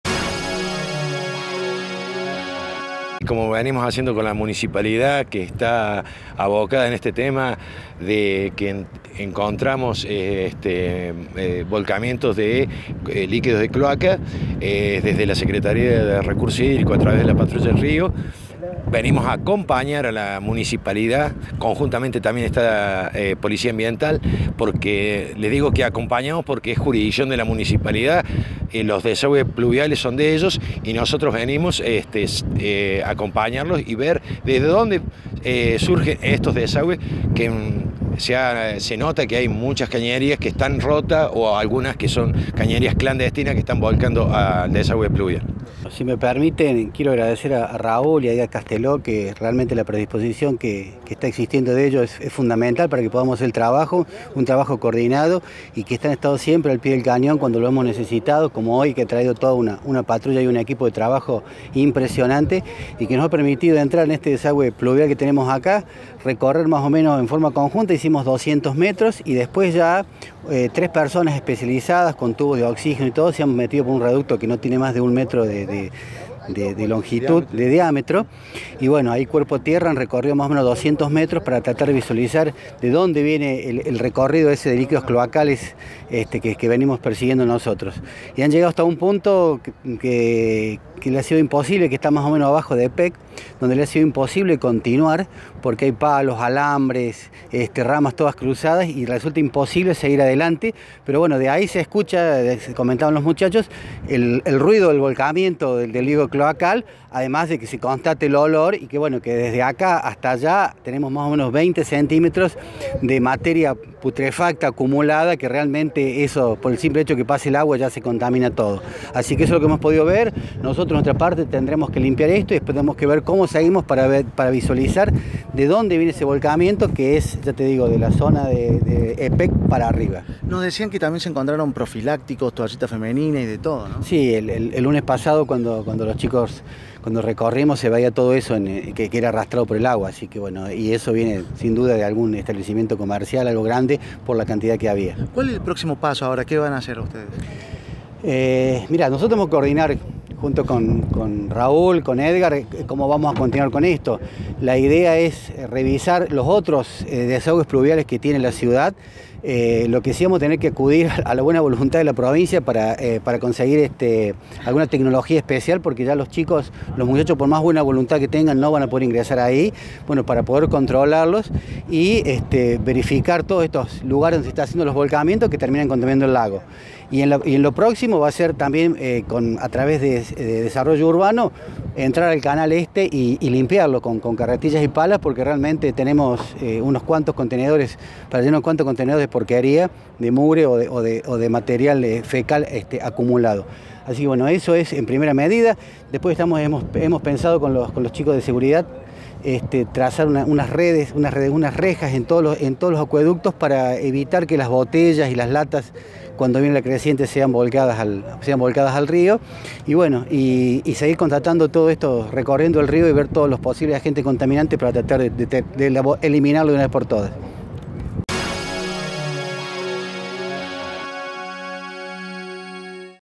Continúan las inspecciones en los desagües de Carlos Paz
Este miércoles por la mañana continuaron los controles a desagües ubicados en el sector de la costanera, cercano al Puente Cassaffousth. Allí  habían detectado volcamientos de líquidos cloacal.

Las inspecciones están a cargo de  Secretaría de Salud del Municipio junto con la Patrulla del Río, la Policía Ambiental y Bomberos de la Provincia.